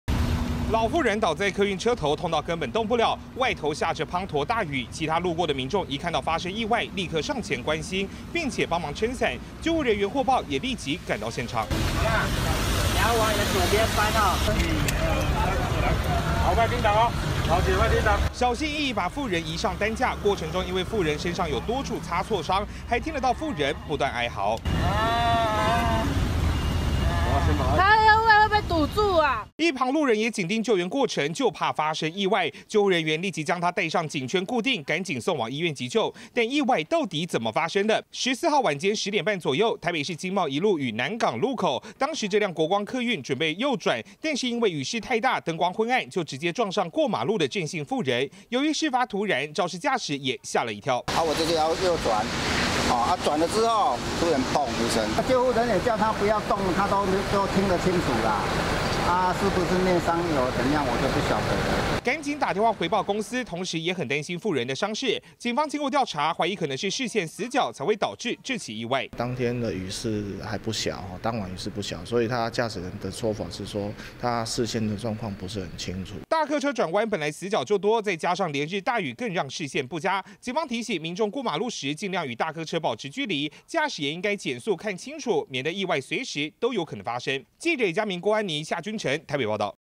20171015中天新聞 大雨阻視線+轉彎死角 客運撞飛老婦
連日大雨不止在各地造成災情，路上交通也是意外頻傳，台北市南港展覽館周邊，14號晚間十點半，一輛國光客運在路口右轉的時候，疑似因為雨勢太大視線不佳，加上又有轉彎上的死角，直接撞上過馬路的婦人，導致婦人被撞飛，躺在地上不斷哀嚎，所幸緊急送往醫院急救，傷勢沒有大礙。